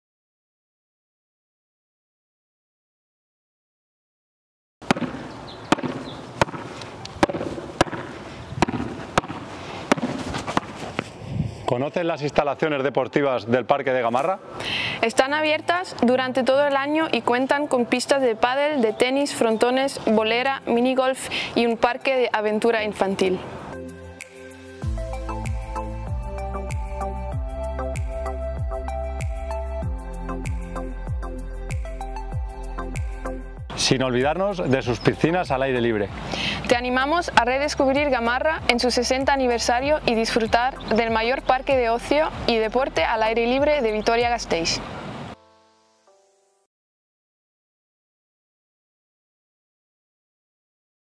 Gamarra 60 urteurrena / 60 aniversario Gamarra - 2024
Uda honetan Gamarrako parkeak 60. urteurrena beteko du. Kike Garcia eta Stefanie Da Eira Deportivo Alaveseko jokalariek ibilbidea egiten dute instalazioetan.
60. urteurreneko jarduerei buruzko informazio gehiago: v-g.eus/gamarra60

Este verano el parque de Gamarra cumple su 60º aniversario. El jugador del Deportivo Alavés Kike García y la jugadora Stefanie Da Eira hacen un recorrido por las instalaciones.
Más información sobre las actividades por el 60 aniversario: v-g.eus/gamarra60